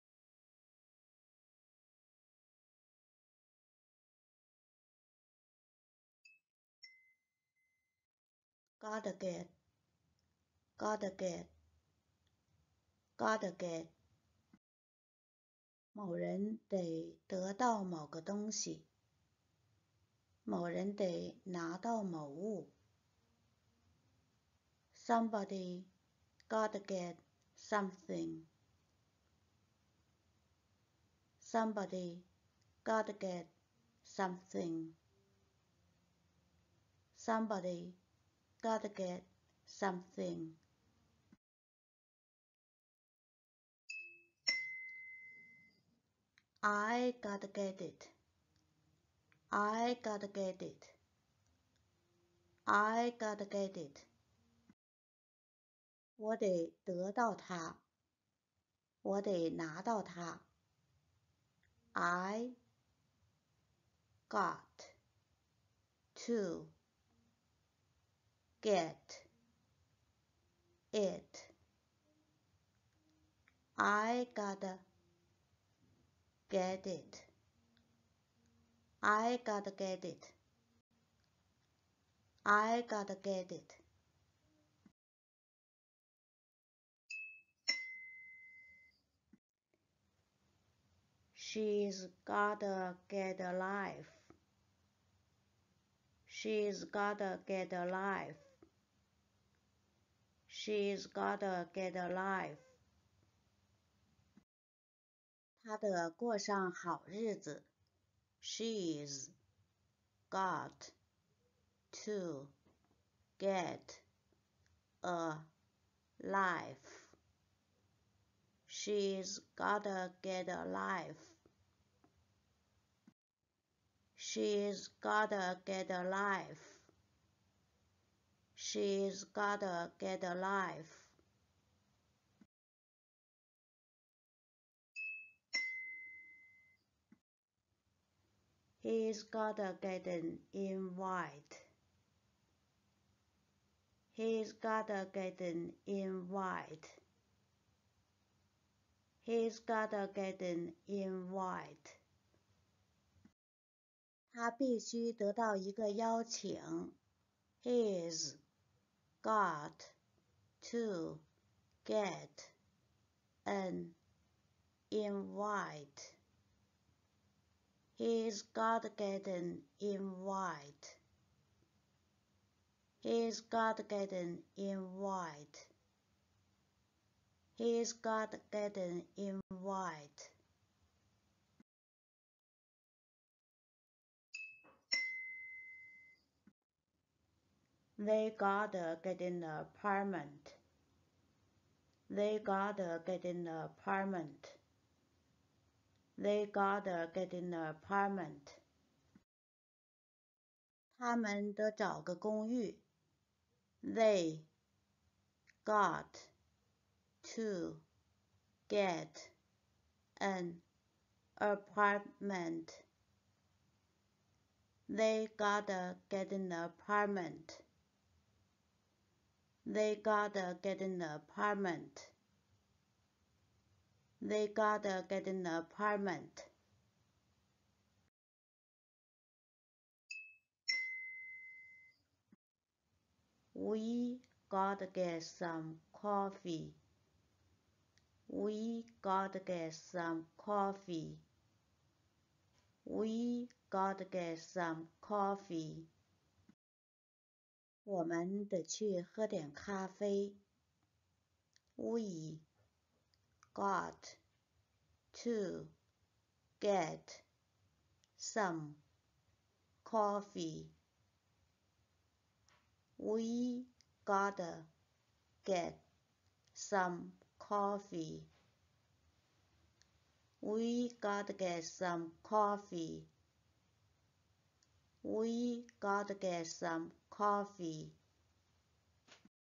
萬能美語句型：第五課｜讓你的英語更流暢｜零基礎英語
1、萬能美語句型：第五課｜讓你的英語更流暢｜零基礎英語口語

2-精選
這 5個句子是為英語初學者精選 ，陸續我們會推出系列萬能句型，每期保持讀者的高效注意力，時間控制在 20分鐘左右。

3-美式發音，聽力與跟讀配合。

4-慢速朗讀，便于學習。
一遍中文，三次英語，加深印象，聽著聽著就會了。

5-這個視頻適合英語入門者，ESL的學生，英語初學者，学TOFEL托福，雅思的同學。

制作一個這樣的視頻要花十多個小時 的工作，如果對你的學習有幫助，請訂閱點贊支持我，也請分享給需要的人！非常感謝你！